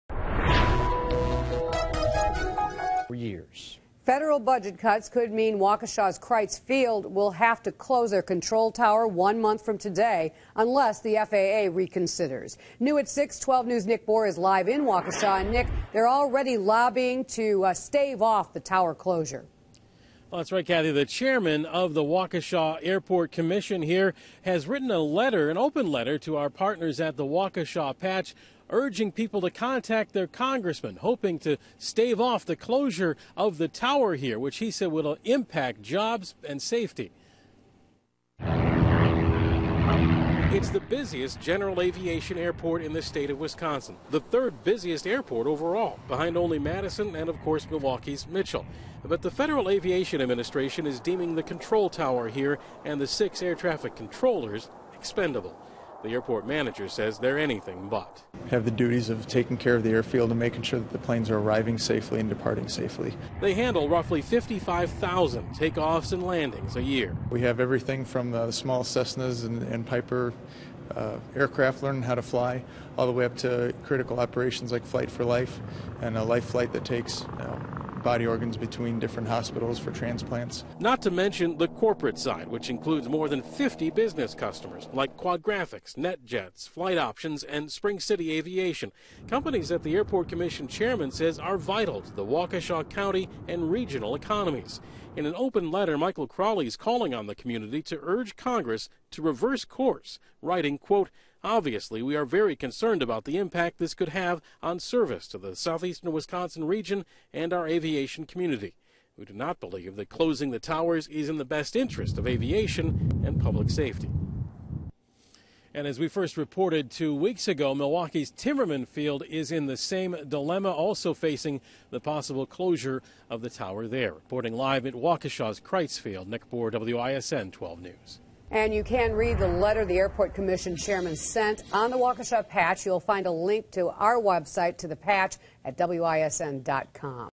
Control tower at Crites Field may shut down
The control tower at Crites Field in Waukesha may shut down in April.